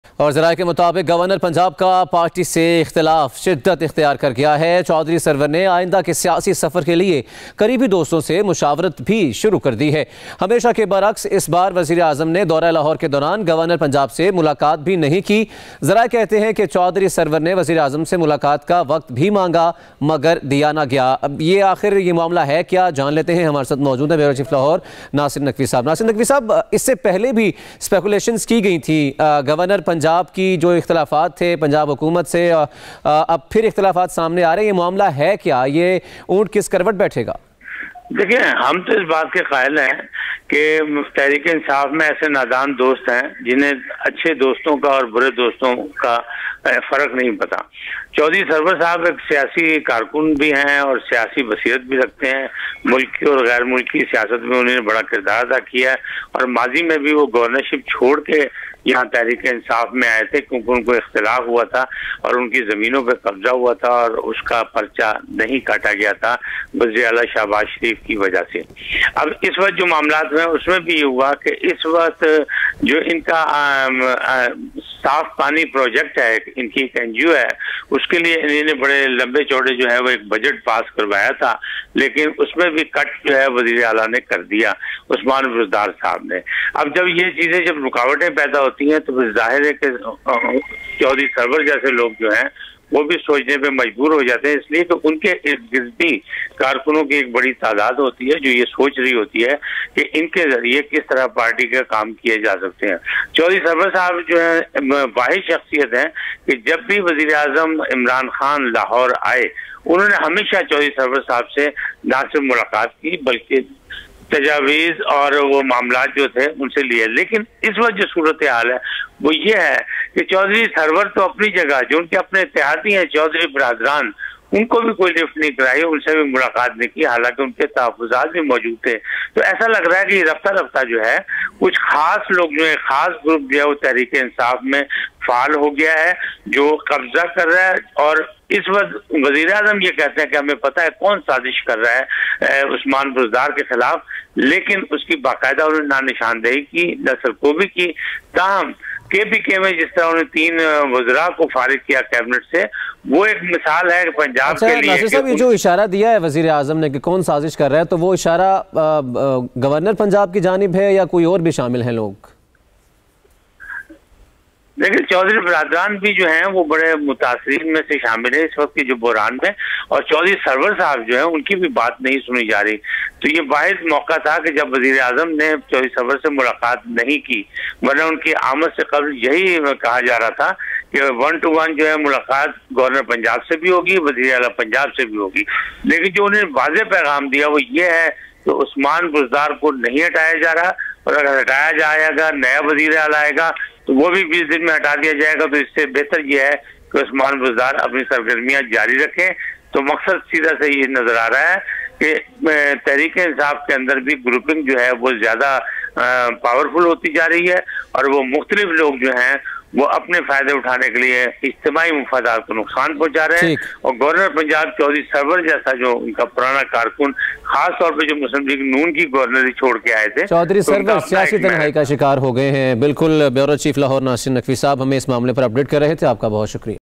Differences between PTI and Governor Chaudhry Muhammad Sarwar
Differences between PTI and Governor Chaudhry Muhammad Sarwar | PM Imran Khan did not meet him on Lahore visit

Team Members:
Ehtisham ul Haq | Farha Sadia | Sundas Khan | Usama Tayyab | Zamir Haider | Adnan Haider